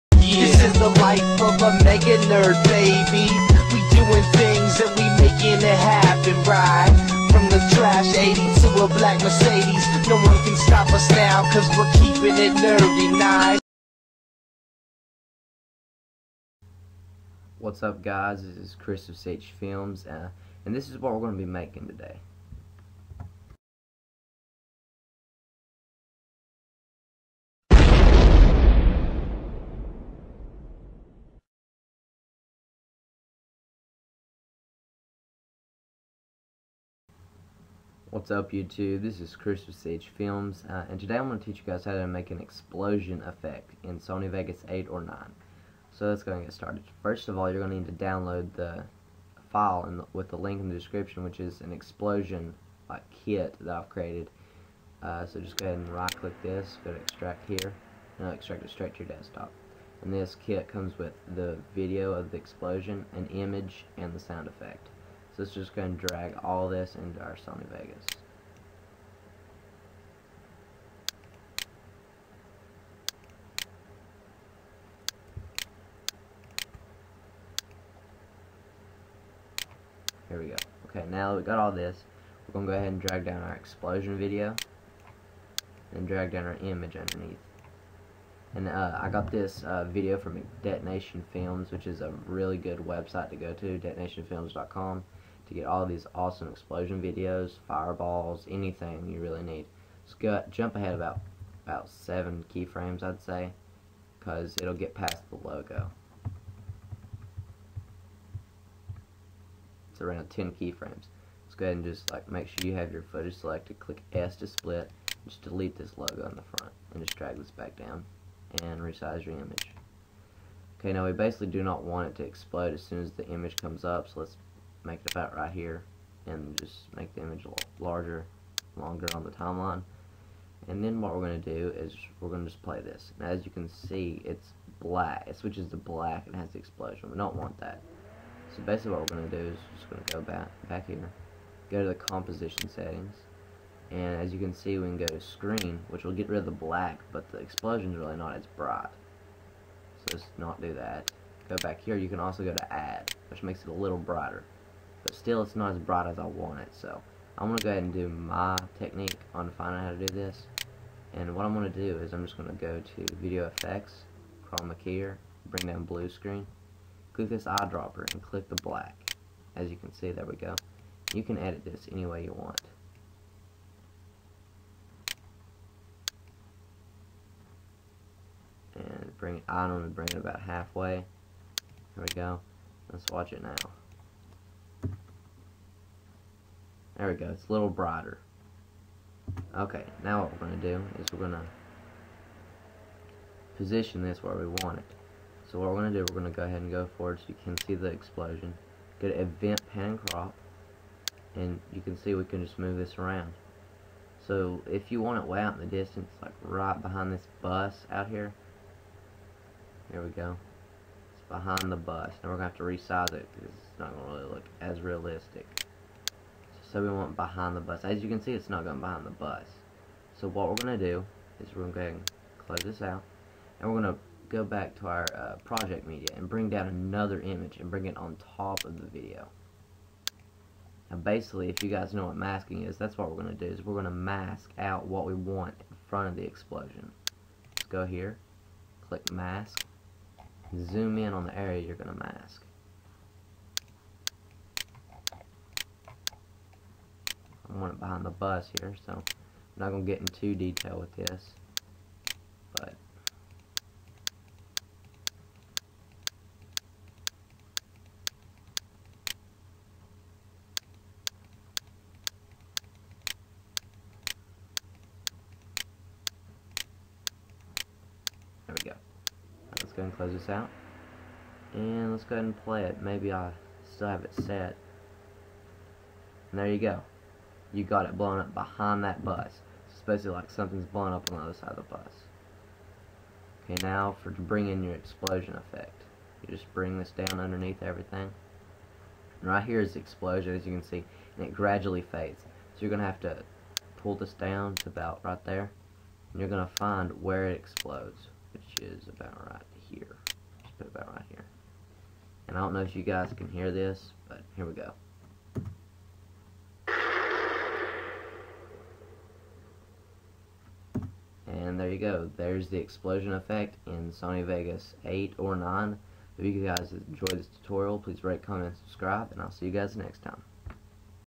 Explosion - Sony Vegas Pro 8
Hey Guys...In This Tutorial I Will Show You Guys How To Create An Explosion. I Haven't Made A Tutorials In A While Because MW2 Came Out But Anyway Here It Is.